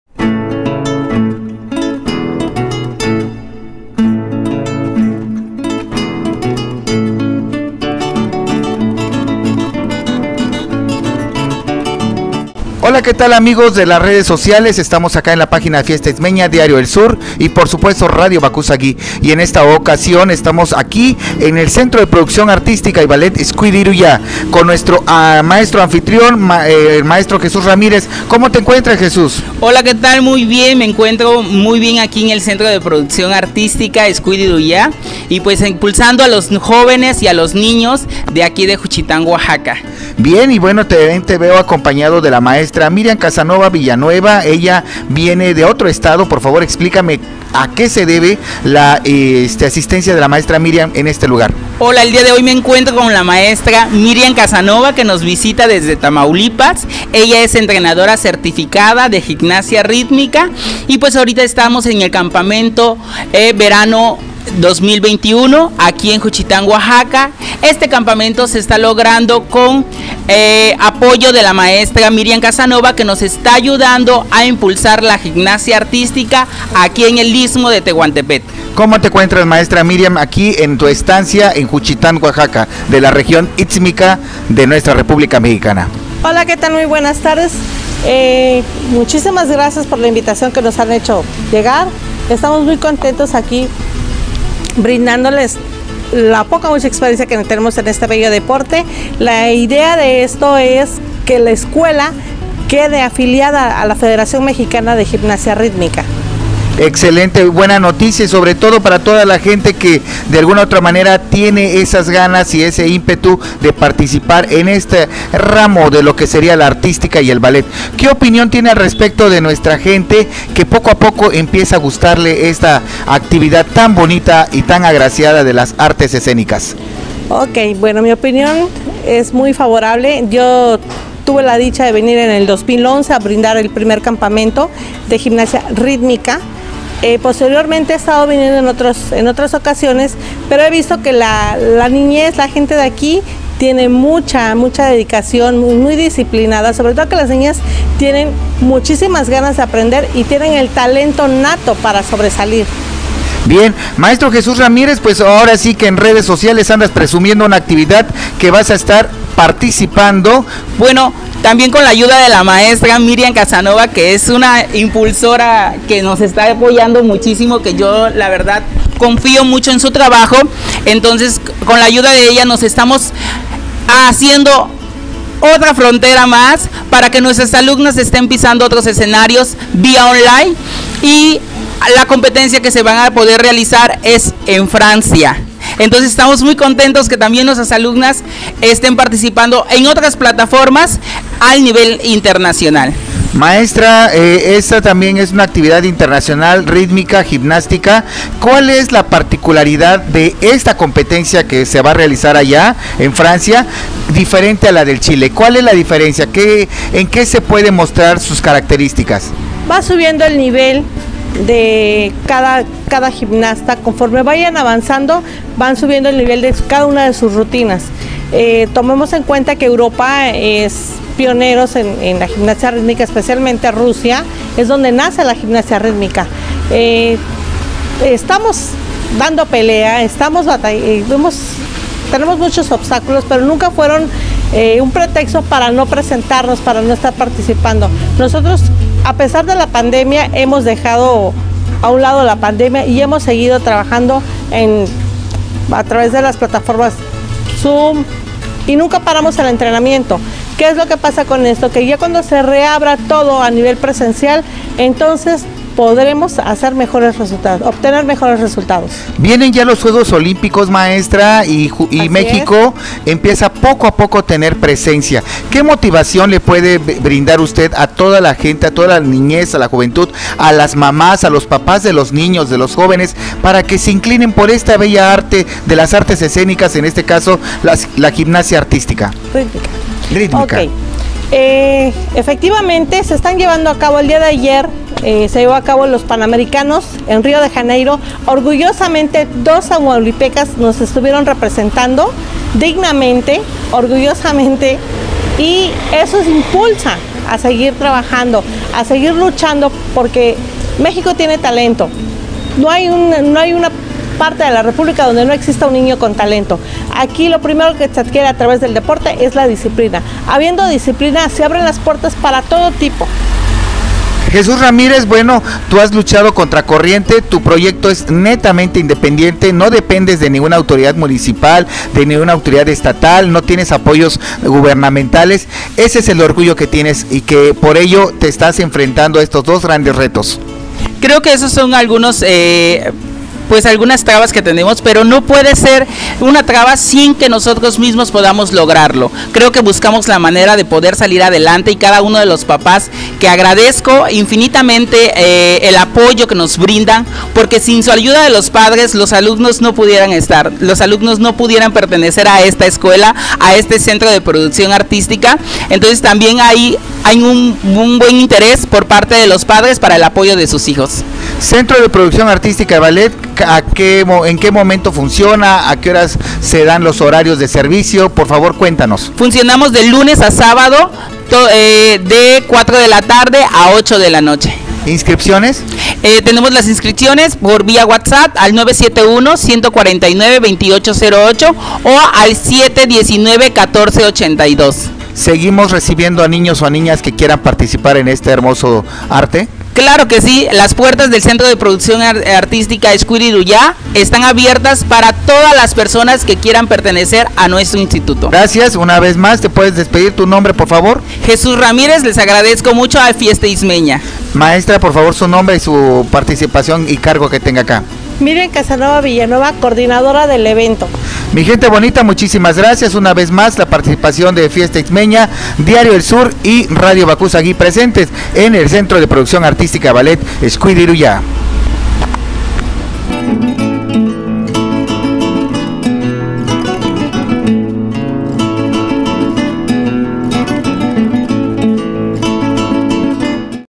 Centro de Producción Artística y Ballet “Xcuiidi Ruyaa”
Centro de Producción Artística y Ballet “Xcuiidi Ruyaa” del Profesor.  Jesús Ramírez
Conociendo a un más de la Gimnasia Artística con la Maestra. Miriam Casanova

Estarán presentes en el SPORT ADHESIF CUP International Rhytmic Gymnastics Online Tournamet
Este 14 y 15 de julio